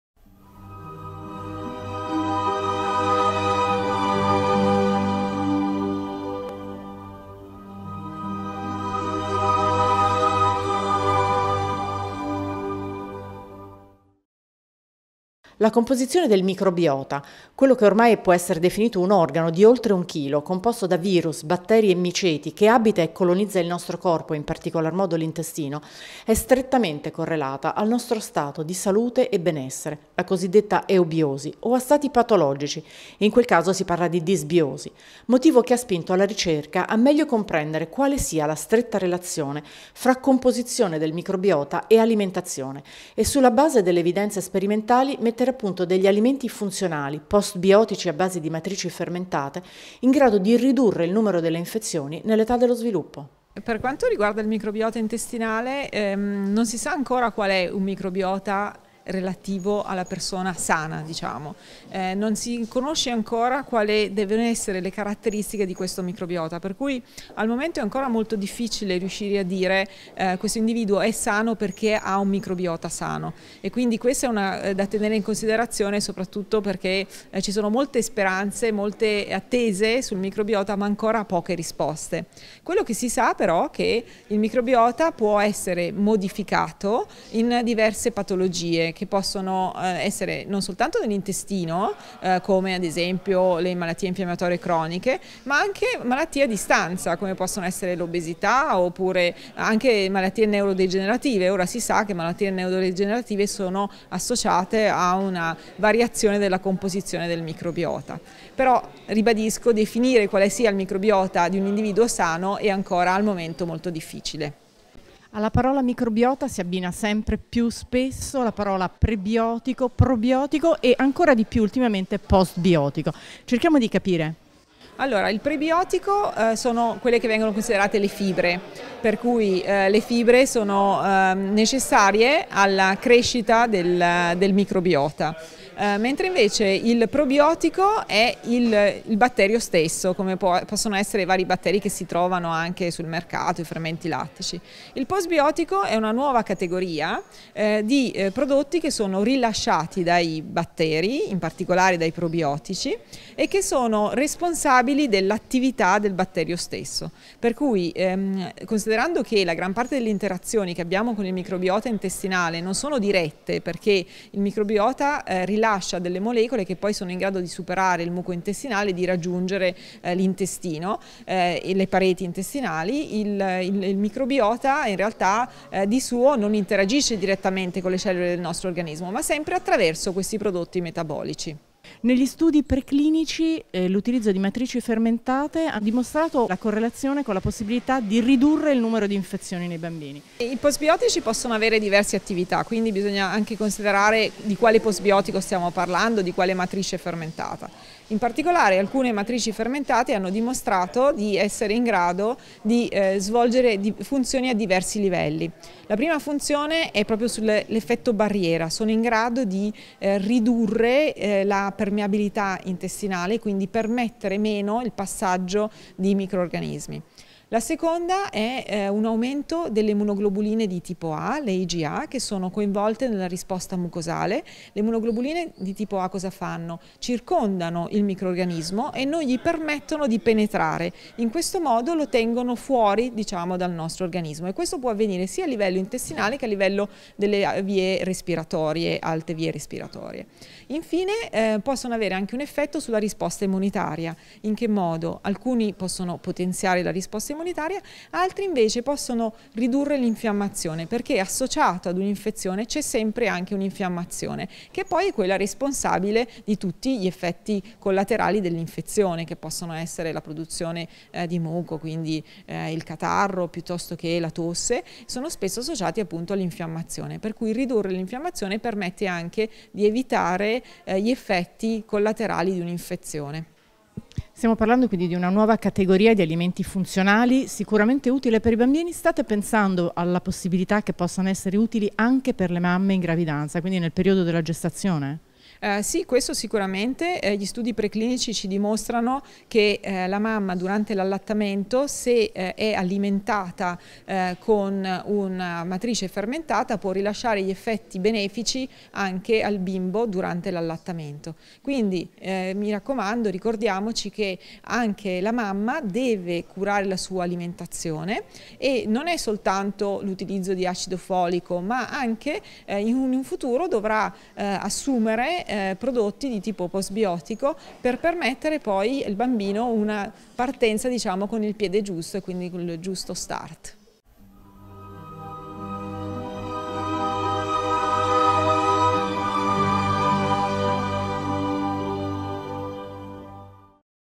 Nuove evidenze dalla ricerca sul Microbiota dei bambini e sul ruolo degli alimenti funzionali
La composizione del microbiota, quell’organo di oltre un chilo composto da virus, batteri e miceti che abita e colonizza il nostro corpo e in particolar modo l’intestino è strettamente correlata al nostro stato di salute e benessere, la cosiddetta eubiosi o a stati patologici, si parla in questo caso di disbiosi, motivo che ha spinto la ricerca a meglio comprendere quale sia la stretta relazione fra composizione del microbiota e alimentazione e sulla base delle evidenze sperimentali mettere a punto degli alimenti funzionali, postbiotici a base di matrici fermentate, in grado di ridurre il numero delle infezioni nell’età dello sviluppo. E per conoscere meglio le ultime evidenze negli studi clinici sul microbiota abbiamo incontrato la Prof.ssa Maria Rescigno,  Humanitas University ed Humanitas Research Hospital,  presente al Progetto Academy dedicato alla nutrizione in epoca neonatale.